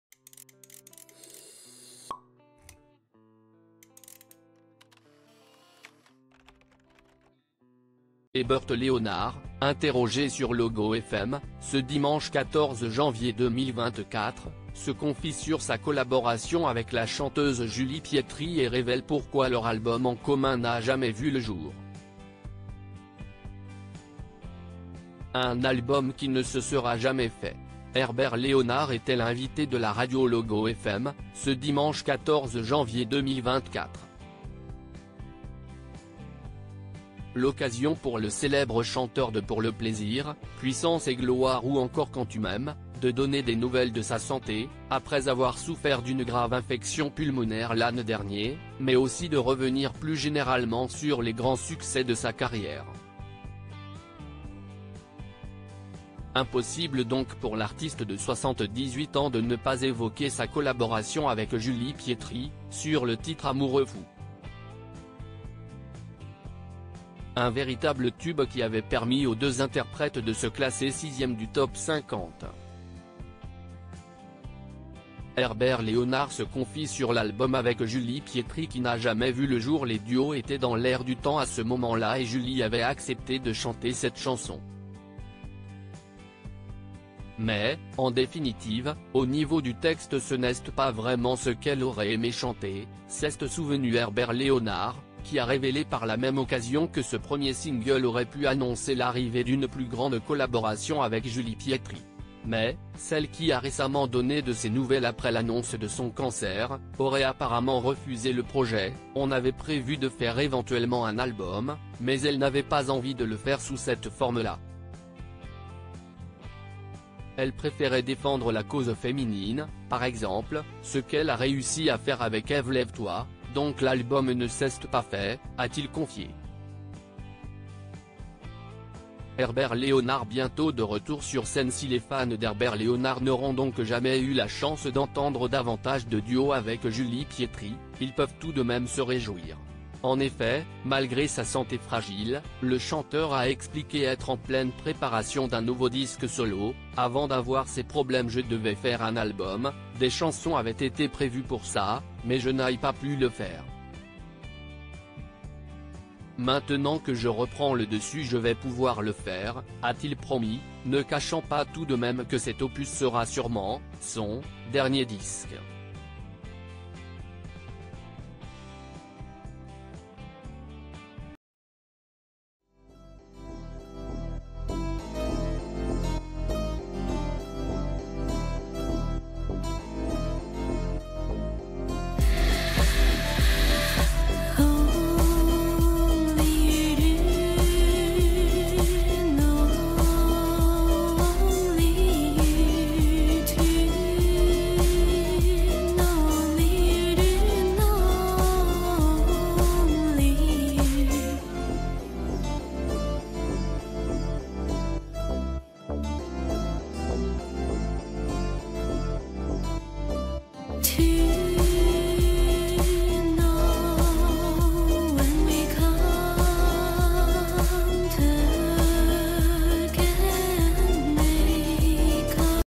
Ce n’est pas ce qu’elle aurait aimé Herbert Léonard revient sur l’échec de sa collaboration avec
"Ce n’est pas ce qu’elle aurait aimé" : Herbert Léonard revient sur l’échec de sa collaboration avec Julie Pietri